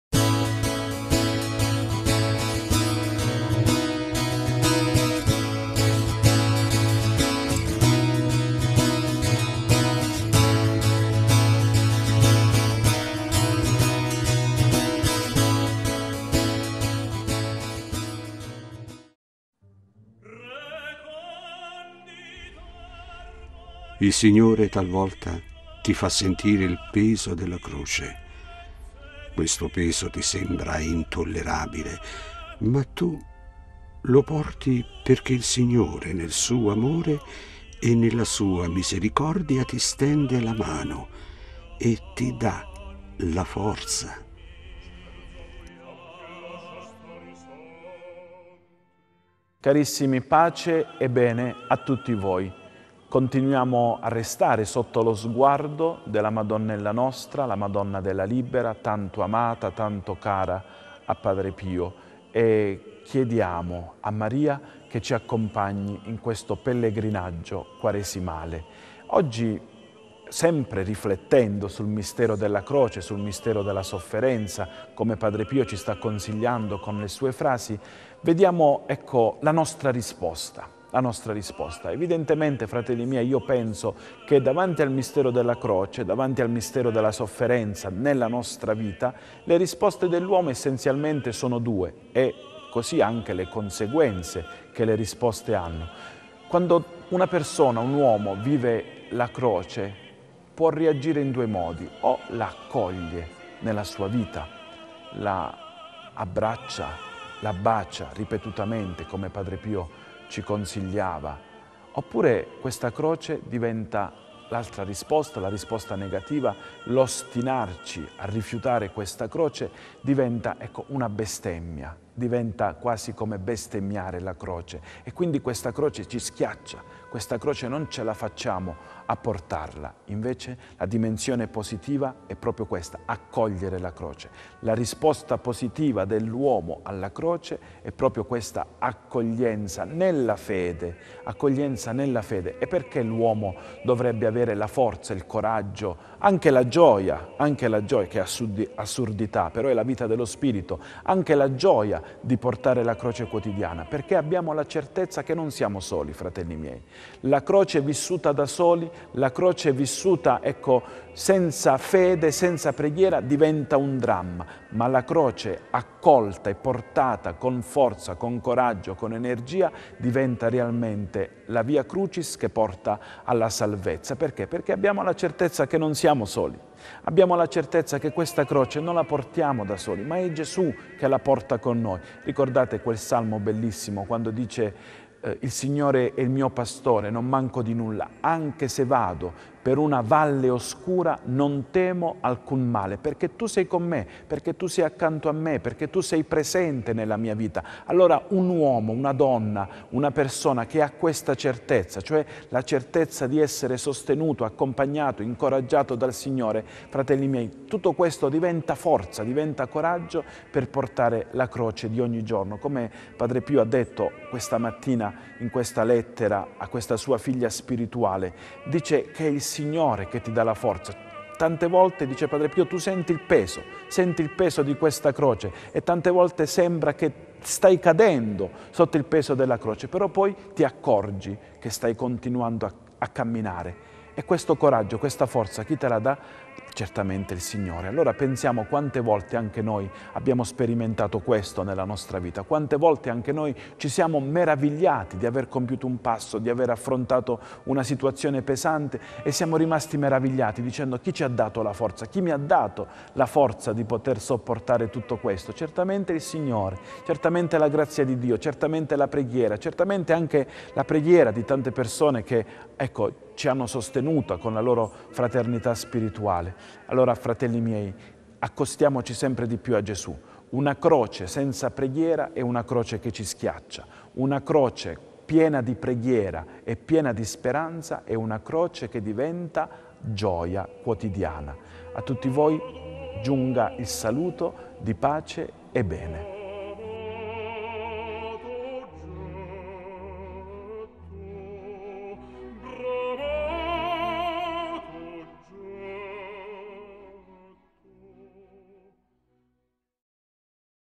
Buona Giornata con Padre Pio 10 Marzo 2020
La Frase di Padre Pio:
"Il Signore talvolta ti fa sentire il peso della croce. Questo peso ti
sembra intollerabile, ma tu lo porti perché il Signore nel suo amore e nella sua misericordia ti stende la mano e ti dà la forza"